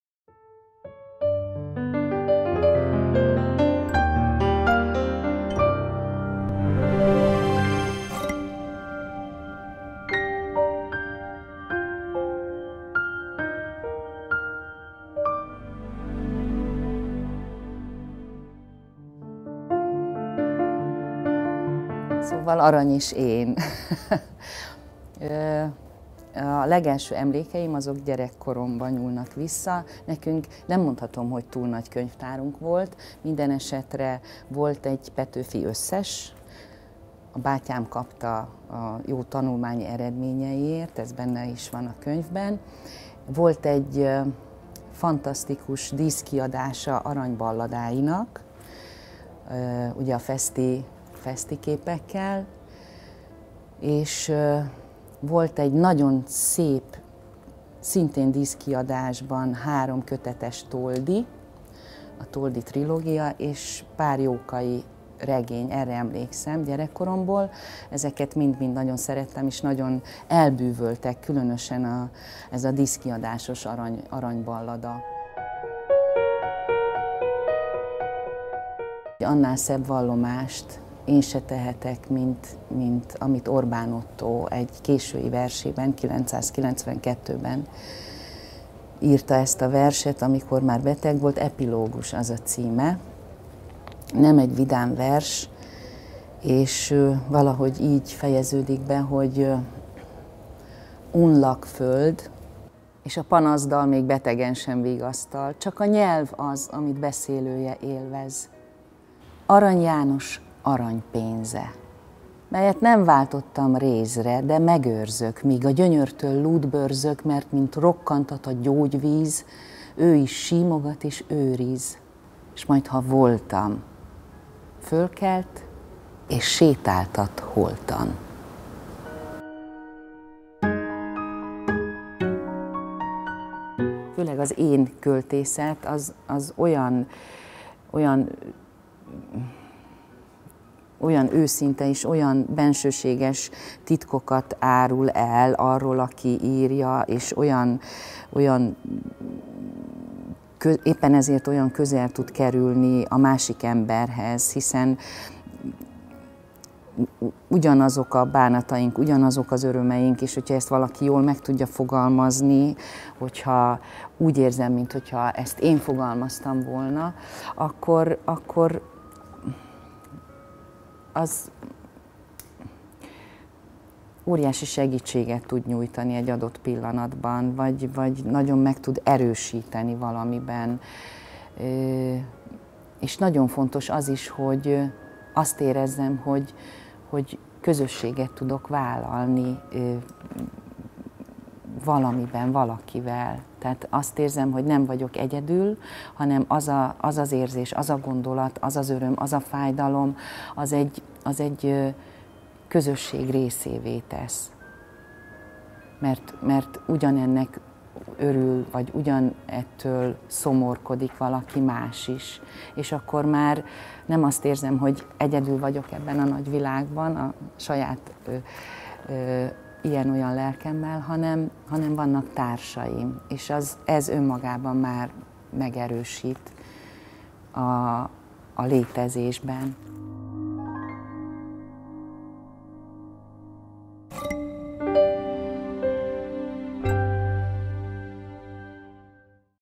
Hallgatni Aranyt! Arany-belépő Ráckevei Annával
Az MMA Hallgatni Aranyt! elnevezésű projektjének keretében a költőfejedelem 130 költeményének és 70 levelének hangzóanyaga kerül fel a program internetes oldalára. A műveket közel 100 színész szavalja el, akik kisfilmek keretében személyesen is vallanak arról, mit jelent számukra Arany János költészete.